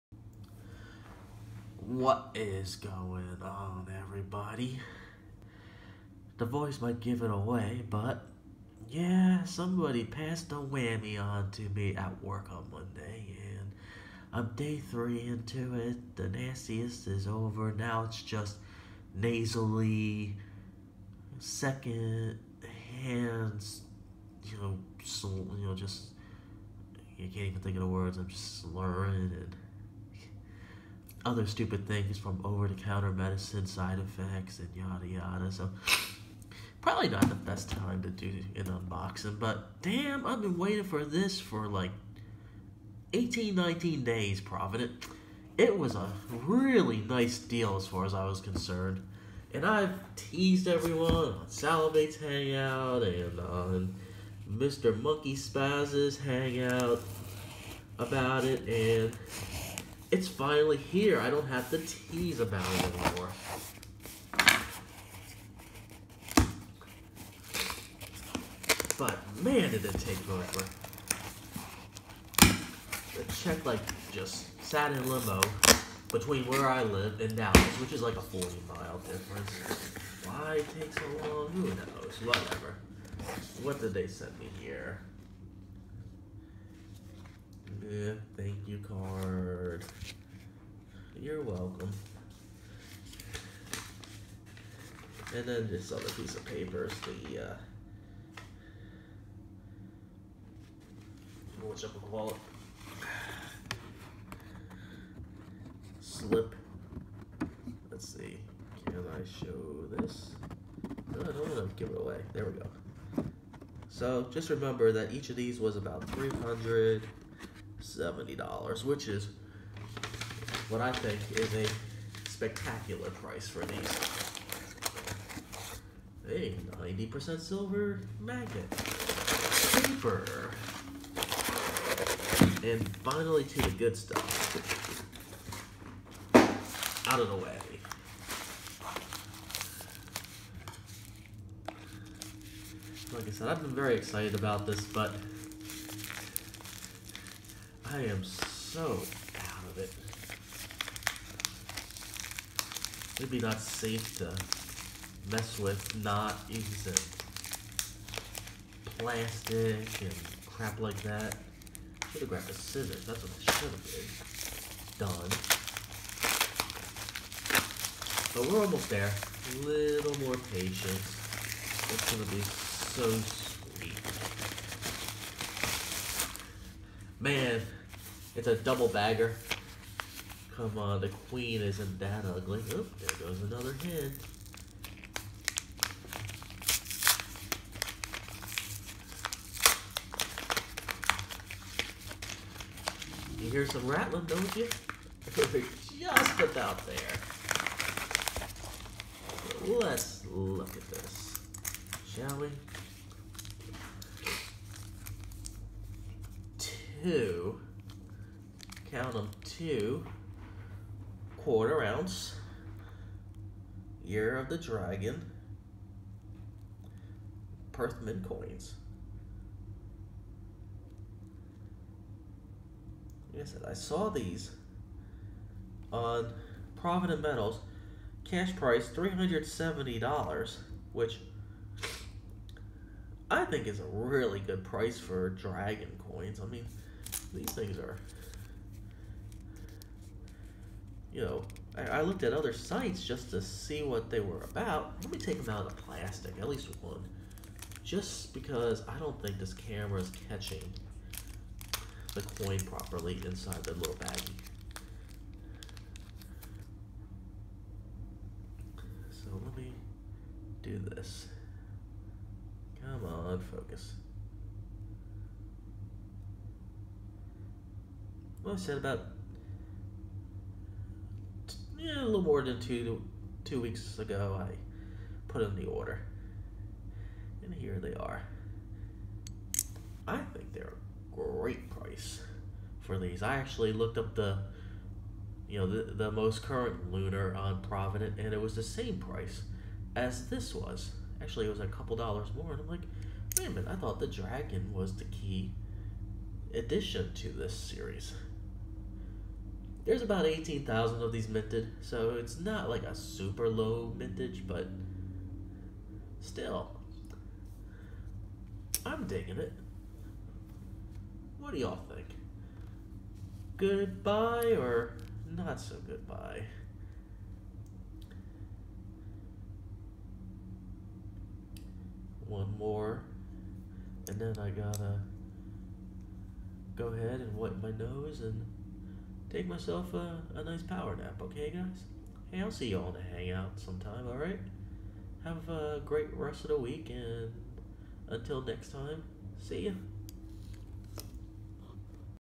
Some key Lunar Gold
I think these at $370 a piece was a super deal.  Well worth the really ling wait from Provident Metals:-)